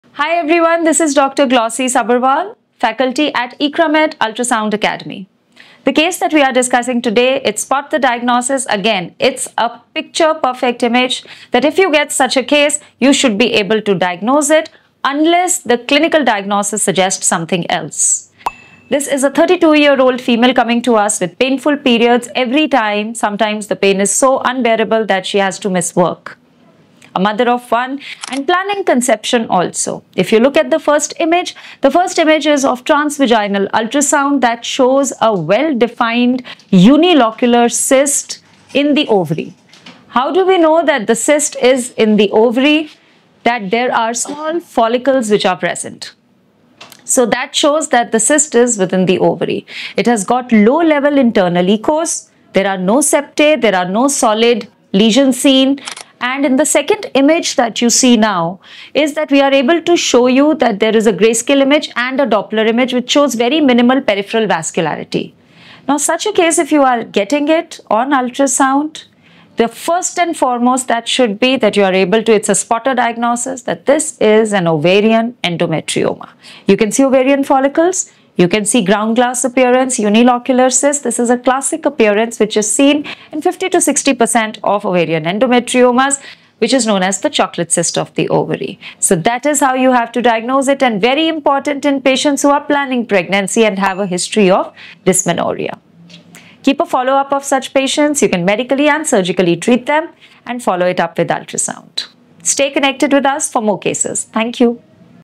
Case 2: Spot the Diagnosis | By Dr. Glossy Sabharwal | Iqramed Acdemy
Description me
Case 2 painful periods

First image shows a well defined avascular unilocular cyst containing low-level, homogeneous "ground-glass" internal echoes, as a result of the hemorrhagic debris. No solid component is seen.

In the second image, there is minimal peripheral vascularity, no intralesional vascular foci seen.
This appearance, most commonly with clinical history of dysmenorrhea suggests endometrioma